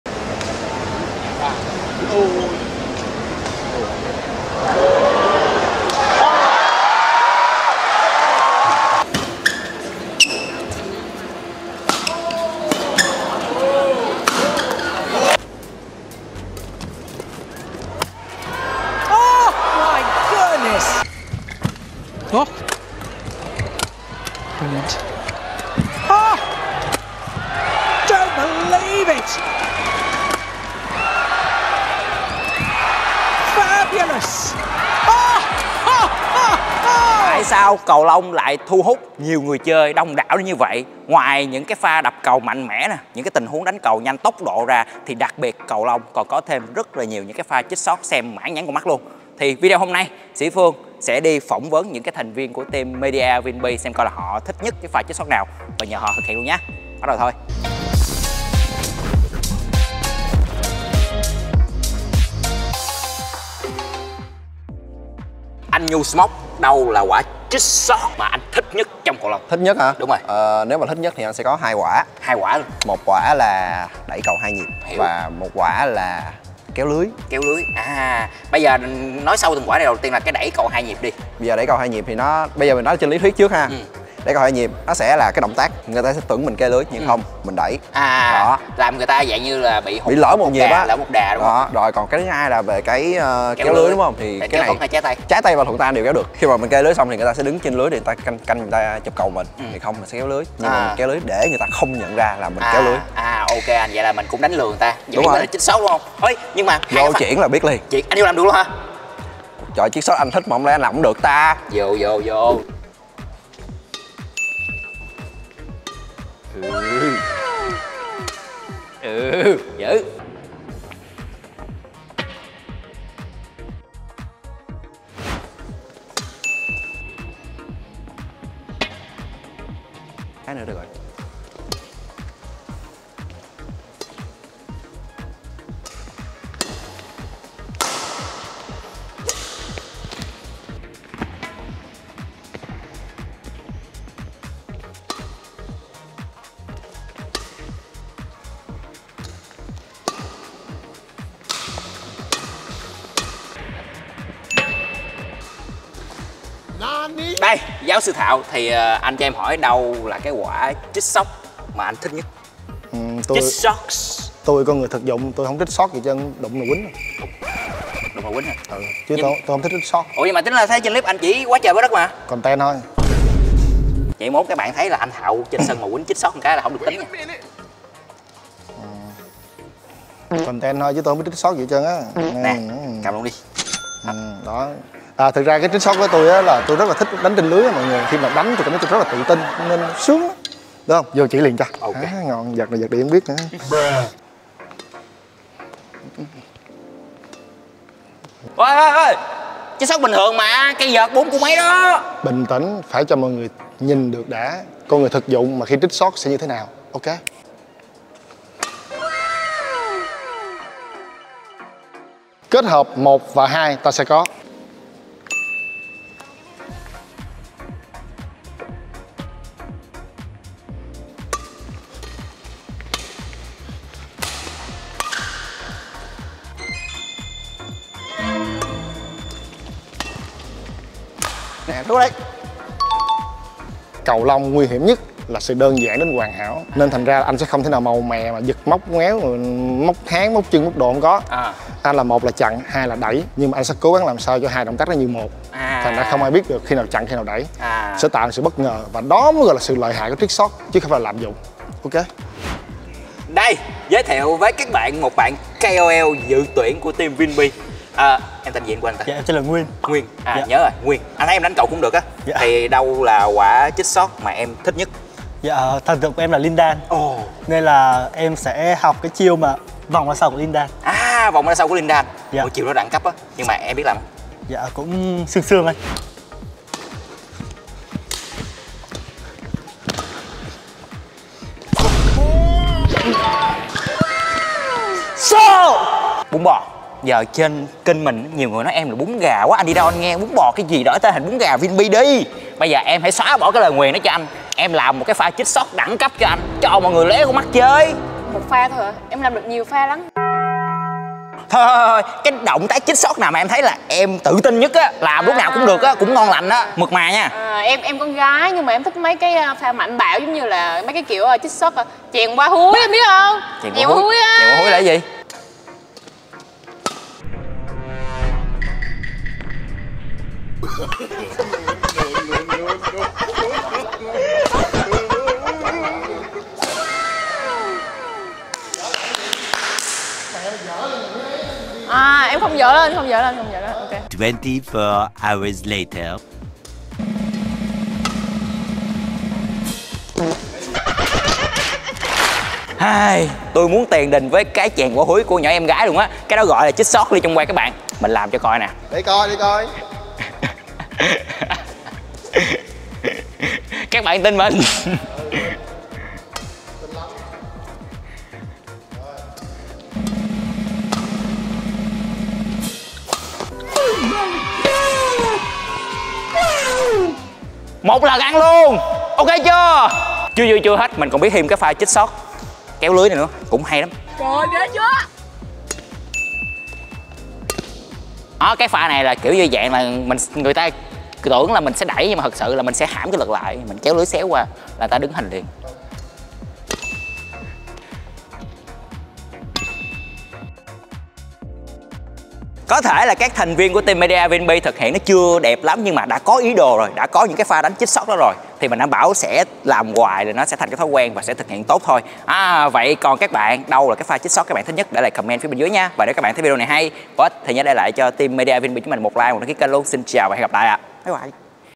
"CHÀNG QUA HÚI" - Khi Team VNB thực hiện TRICKSHOT CẦU LÔNG kiểu...???
TRICKSHOT trong cầu lông là những kỹ thuật lừa cầu lông phổ biến được nhiều người chơi sử dụng để tạo độ bất ngờ, gây bất lợi cho đối phương.

Dựa vào khả năng phán đoán và trình độ chơi thì các vận động viên hoặc người chơi cầu lông đều có thể tạo riêng cho mình 1 pha TRICKSHOT CẦU LÔNG ấn tượng.

Với những thành viên " XUẤT CHÚNG " thì team VNB tất cả đều có những quả TRICKSHOT CẦU LÔNG sở trường  " ĐỈNH NÓC KỊCH TRẦN " " VƯƠN TẦM THẾ GIỚI " !! và tất cả sẽ được bật mí trong video lần này !!

Mời anh em lông thủ cùng xem video, team VNB thực hiện TRICKSHOT sở trường của mình như thế nào  nhé !!

Nếu mọi người quan tâm về những nội dung khác về bộ môn CẦU LÔNG và muốn mình làm Video thì hãy COMMENT phía dưới nhé !